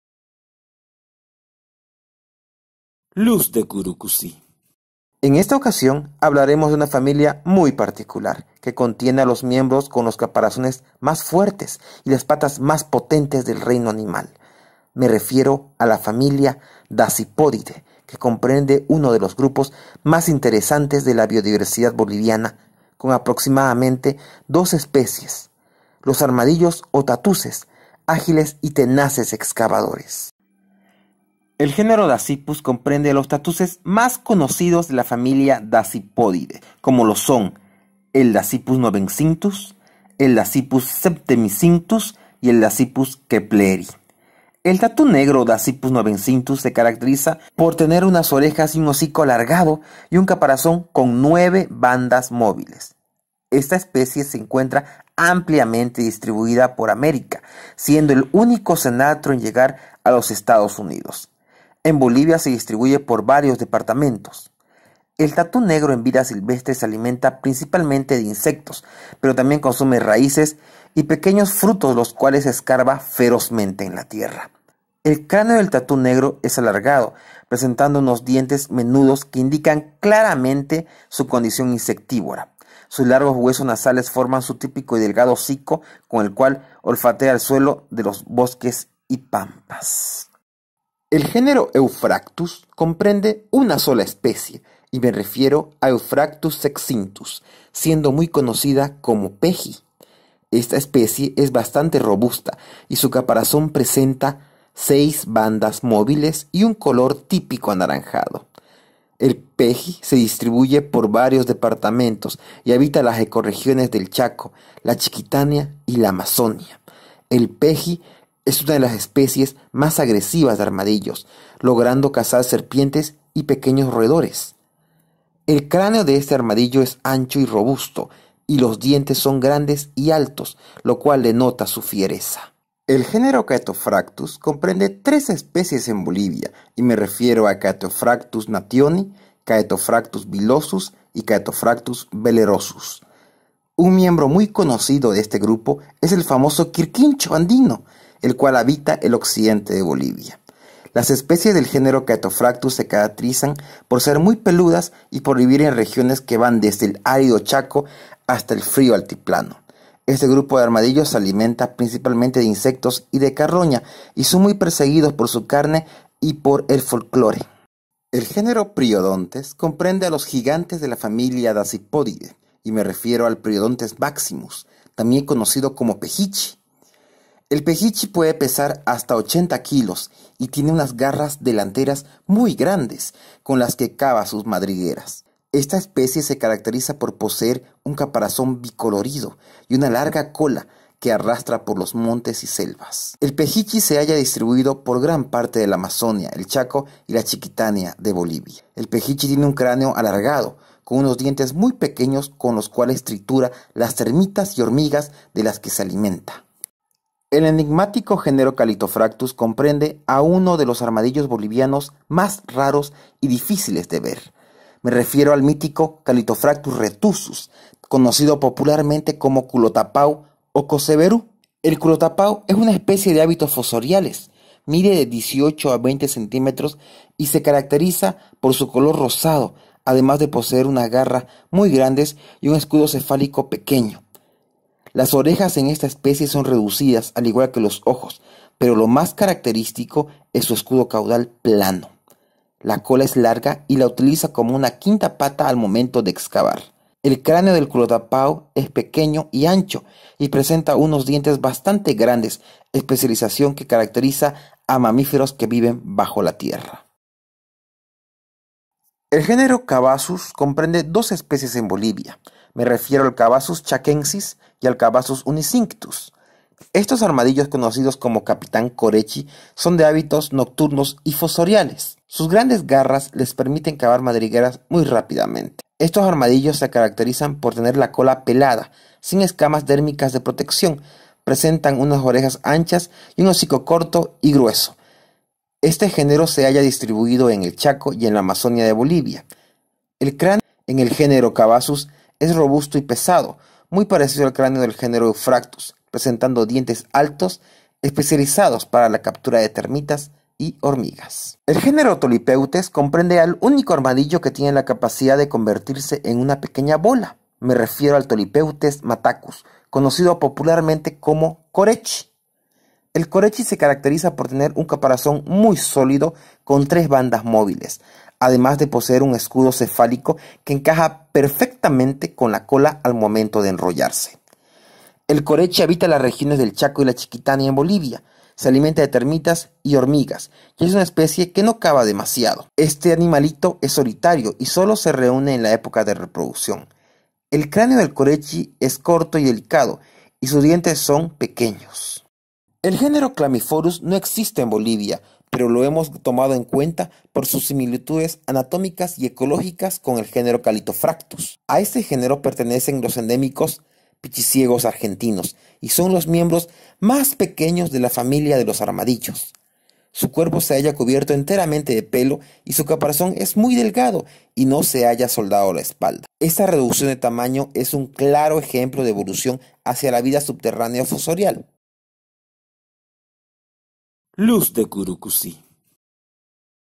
LUZ DE CURUSI Armadillos de Bolivia
El quinto capitulo sobre las especies de armadillos que habitan Bolivia, desde el conocido corechi y el gigante pejichi hasta el enigmatico culotapao rosado.